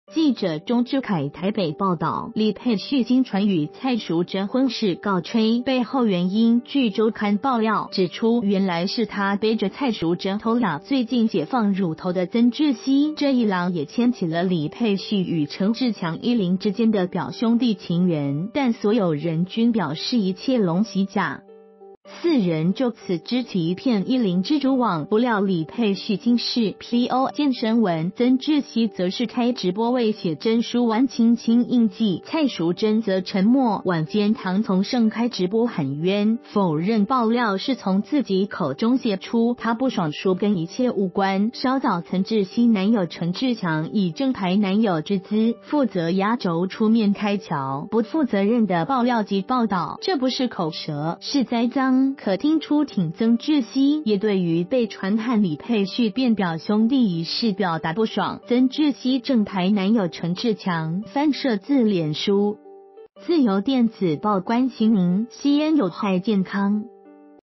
女友喇舌李沛旭淪表兄弟？ 陳志強暴怒開譙
〔記者鍾智凱／台北報導〕李沛旭驚傳與蔡淑臻婚事告吹，背後原因據周刊爆料，指出原來是他背著蔡淑臻偷喇最近解放乳頭的曾智希，這一喇也牽起了李沛旭與陳志強伊林之間的表兄弟情緣。但所有人均表示一切攏洗假。

發表評論評論如下。